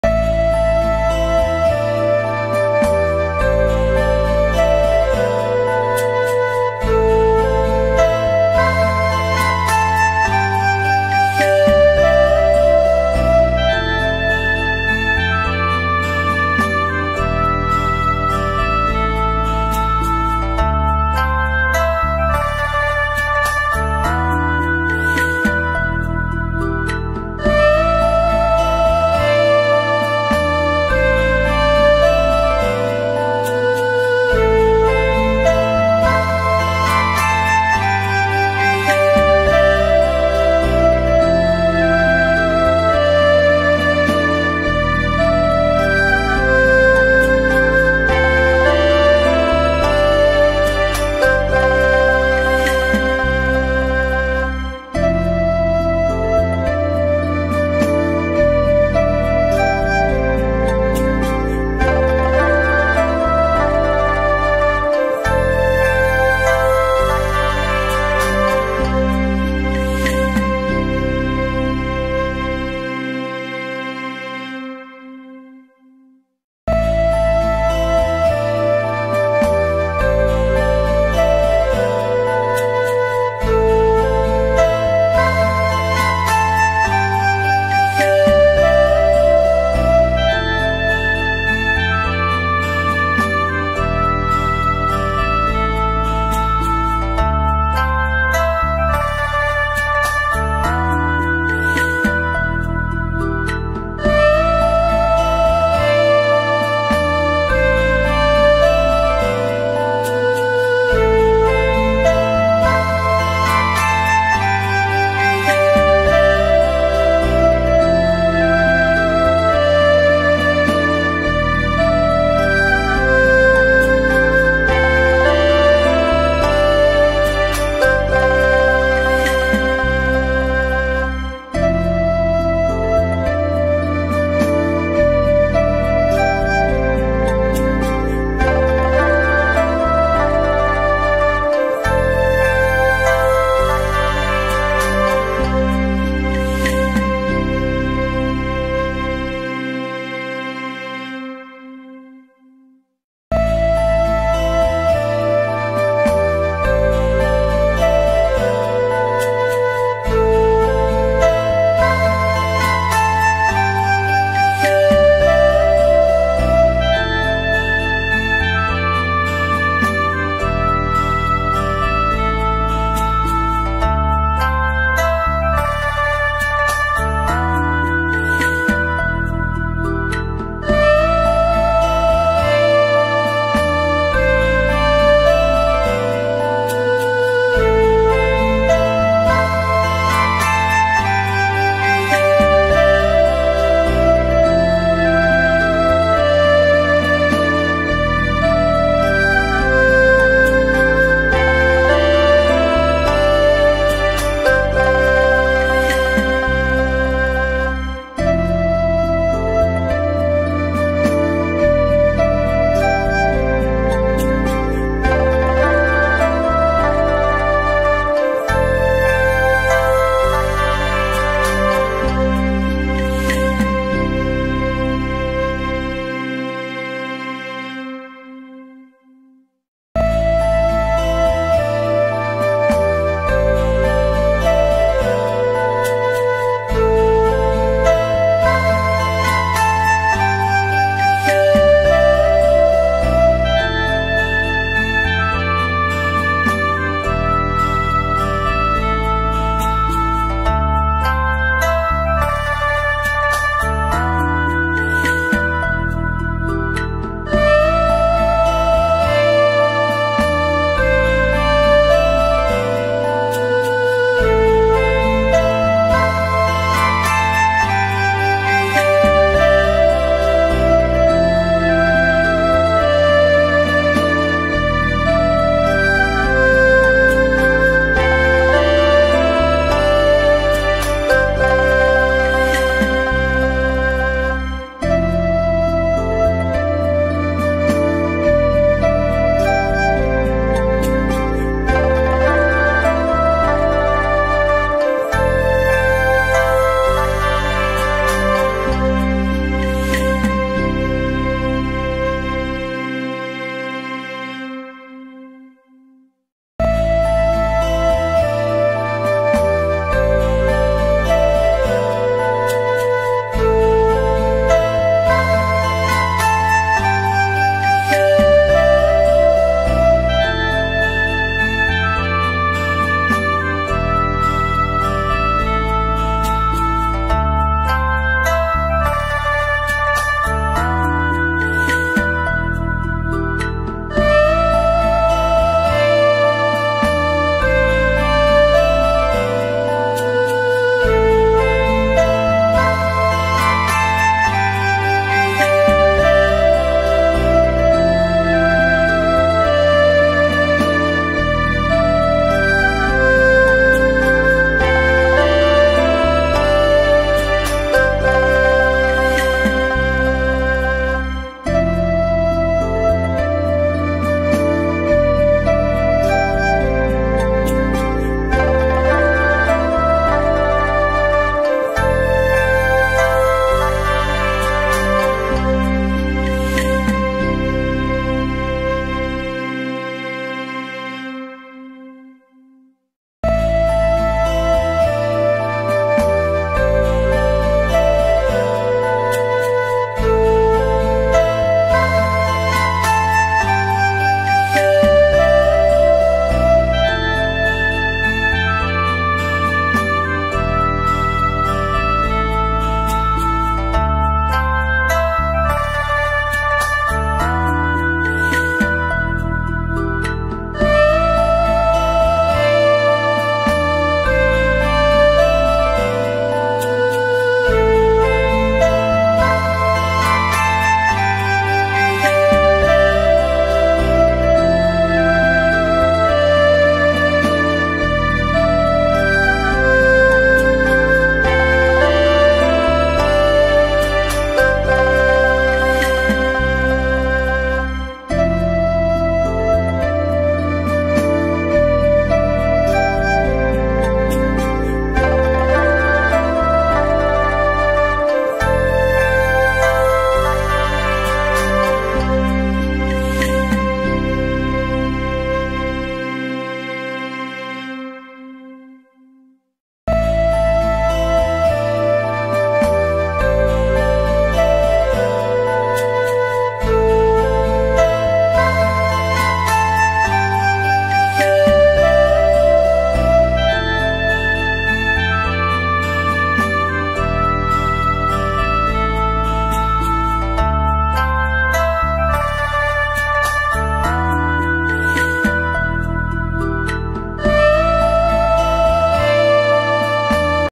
DIY gold lace chain bracelet tutorial, free knitting method sharing
With gold thread and jade thread， it feels like a gold lace for a bracelet. Suitable for girls with many cool clothes.